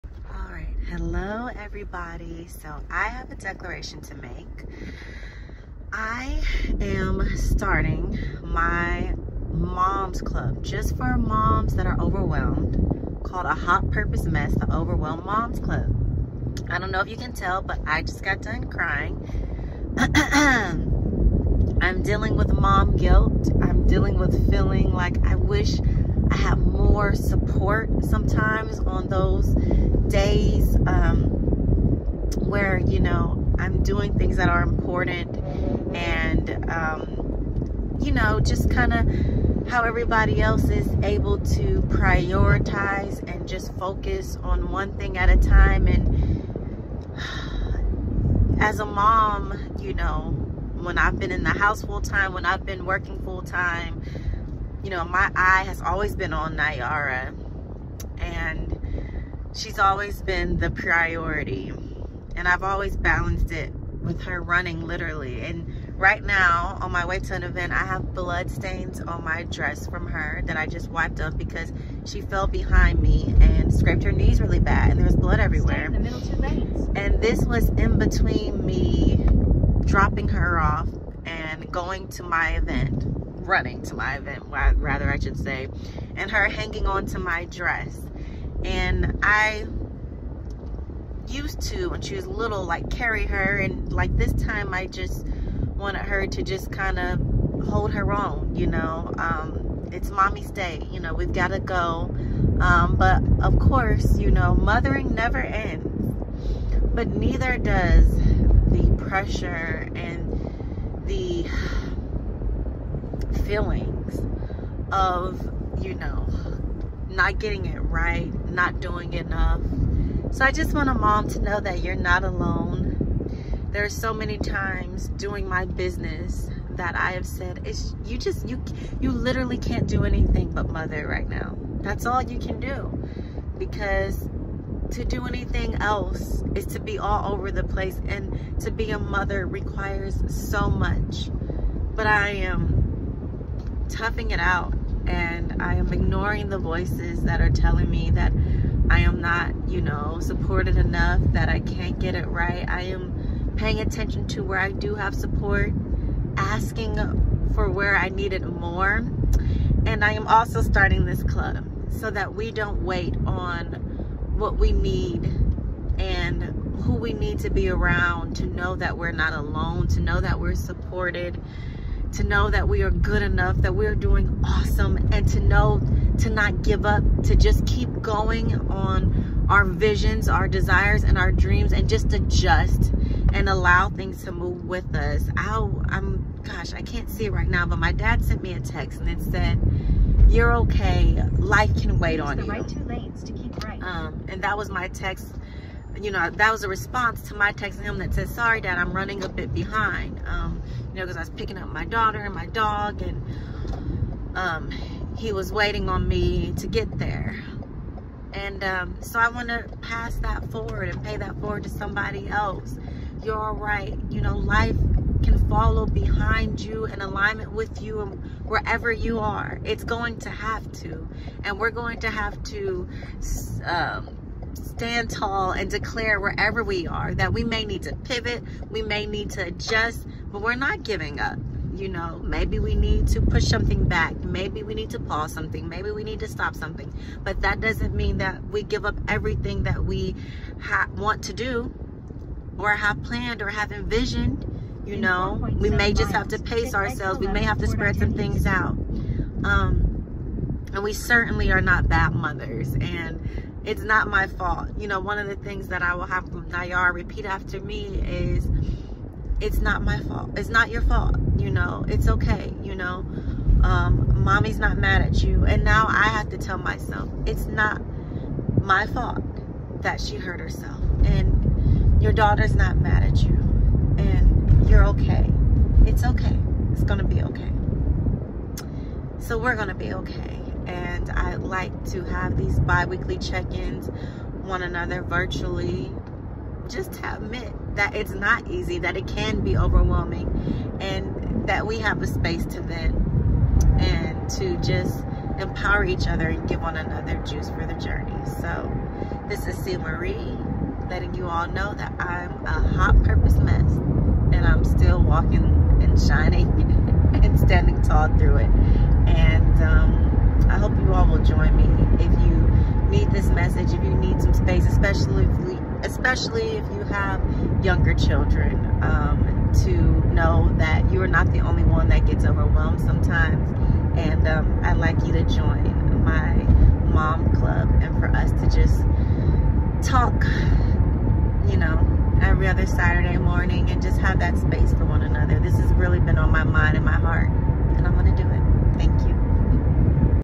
The Overwhelmed Moms Club: Why I'm Saying it Out Loud- Why I'm Starting Our Virtual Club! 🫂💐🥰🎉
Starting my Birthday Month- October🍂- with my 1st Authentic Truth: Motherhood Can be Overwhelming and I Face Overwhelm! 🙋🏾‍♀️
SelfLove is about Honesty and Acceptance- NOT Perfection. SelfCare is Taking Time for Our Needs as Well as Being Affirmed by those Who Empathize and Understand!

Welcome to the OverwhelmedMomsClub! This group is for mothers who are balancing the demands of Living-While-Mothering❣️💯

We are sharing tips, venting about our struggles, listening, loving, stretching and just taking a break from it all. We'll also enhance our self-care, time management, and stress relief!